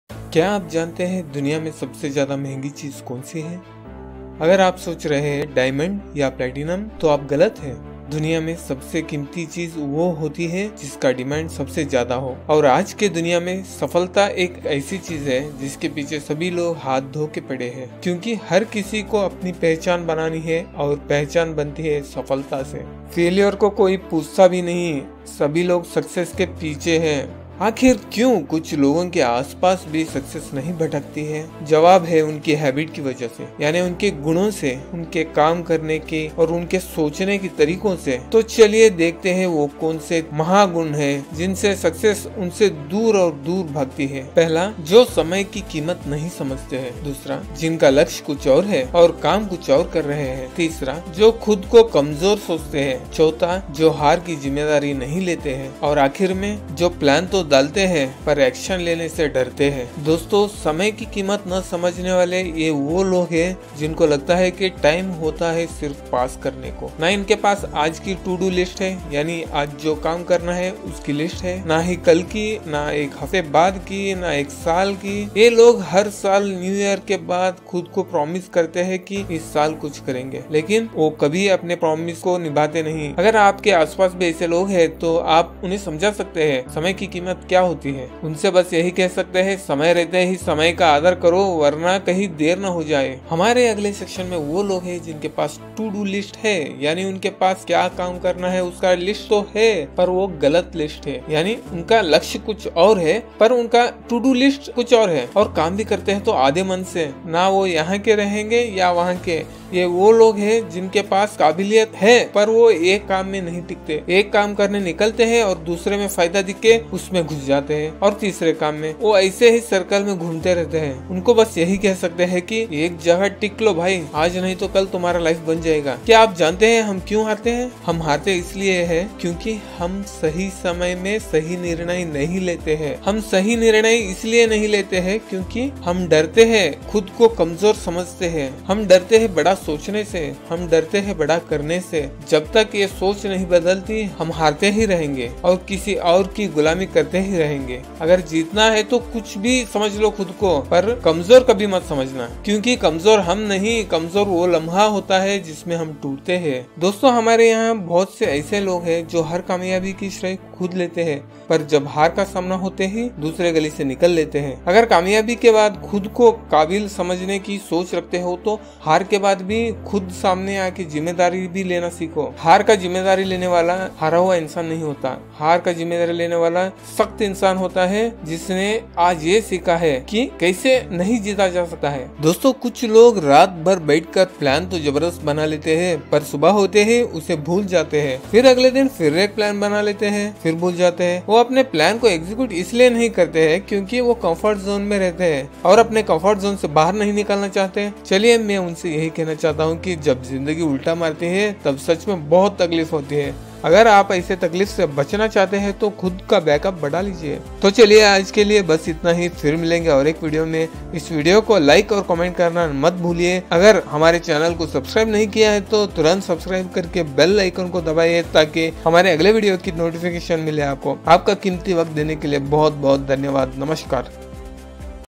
कामयाबी से पांच कदम दूर | 5 STEPS TO SUCCESS
This video tells about th 5 steps that will make you succeed. If you want to get success then follow these steps. A step by step guide to success in hindi. The video is created in hindi language and help youngsters, enterpreneurs and businessman to get success in their life. It tells how people will fail to get success if they do mistakes in these 5 steps.. If you follow and do these 5 steps in positive direction then success is always yours. It motivates the people to get success in life. Motivational video in hindi help all age